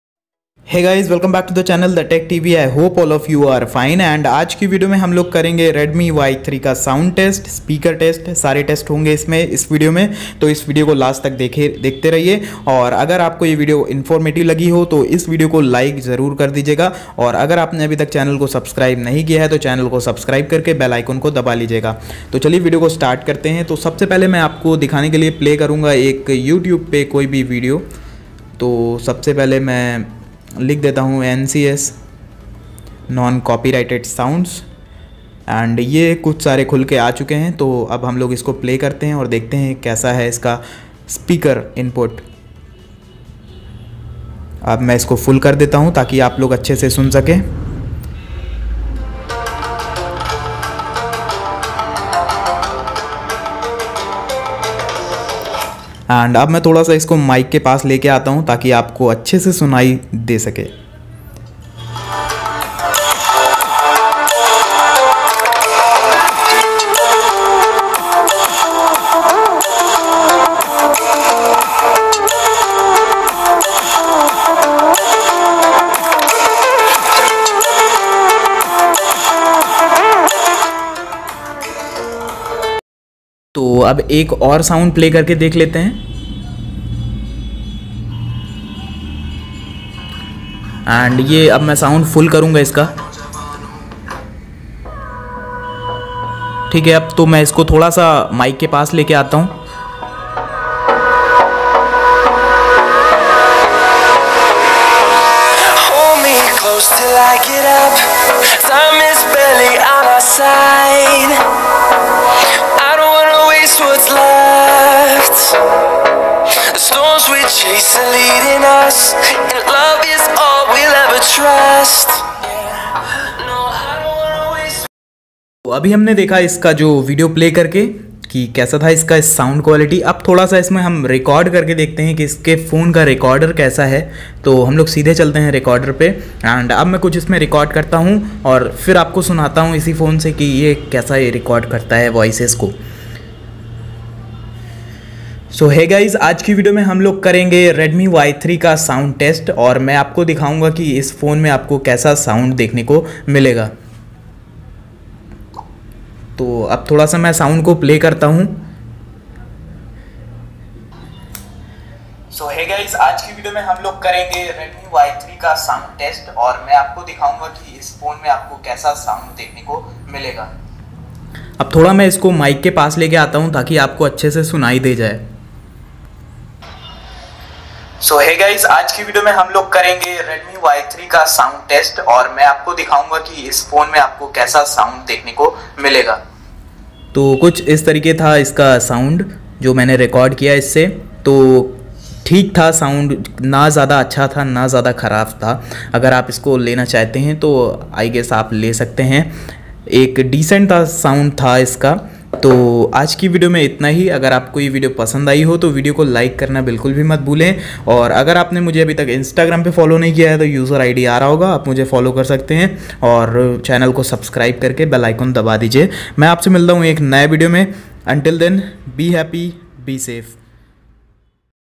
Redmi Y3 Speaker Test (Redmi Y3 Audio Quality Test and Comparision) | Thetechtv
Hi guys welcome back to the channel The tech tv and i hope all of you are doing good and in this video am going to test the speaker and audio of Redmi Y3. I will also record my voice using voice recorder in this phone and will give an review of this Redmi Y3 audio and speaker.

The tech tv
Thetechtv